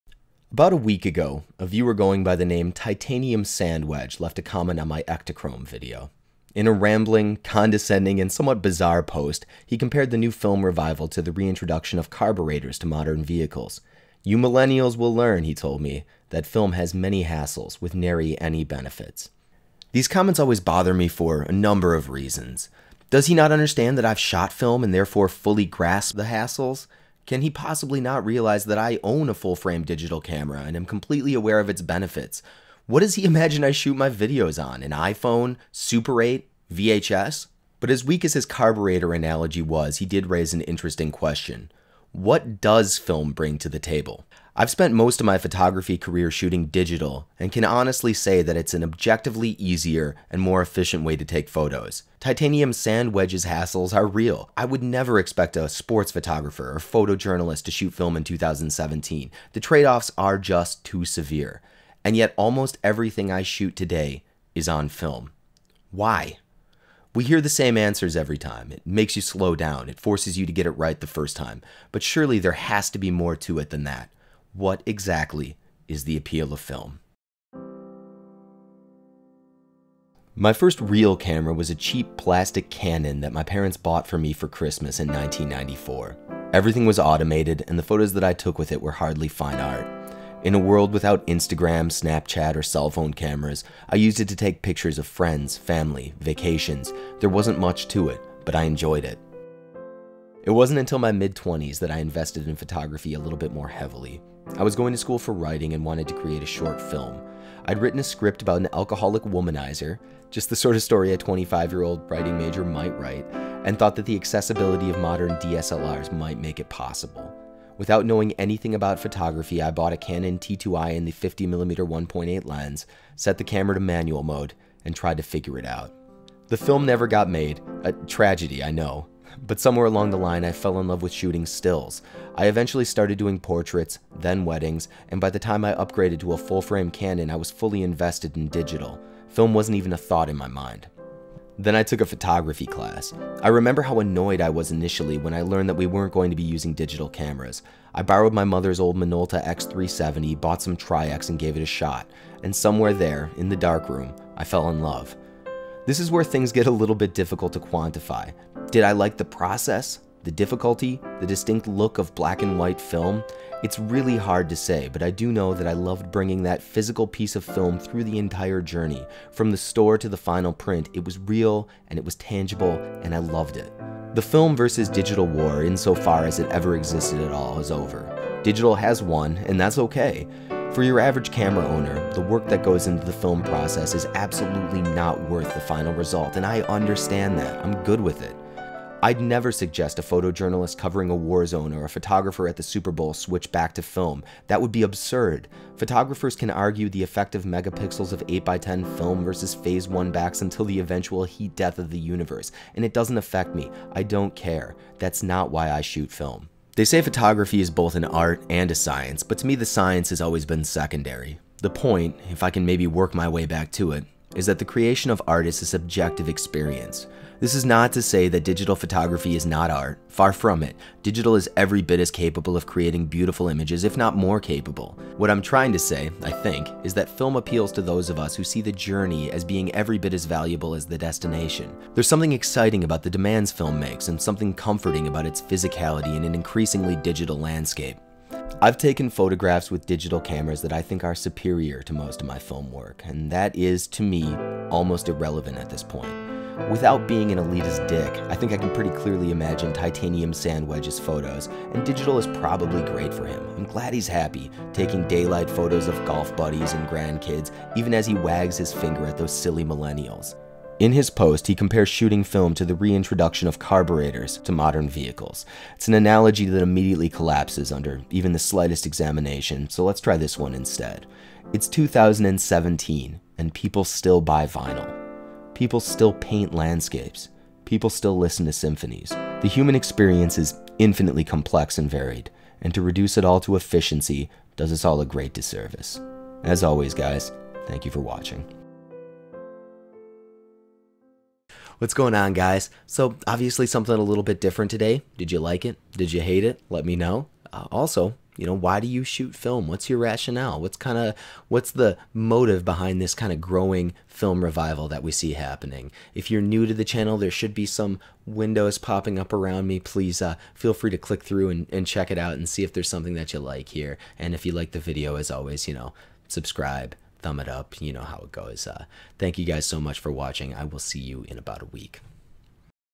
Film vs Digital: Why Choose?
It goes without saying that a channel dedicated to film photography gets the occasional comment asking why I even bother.  One such comment last week got under my skin and prompted a video on the Film photography vs digital photography argument, and why it doesn't really matter.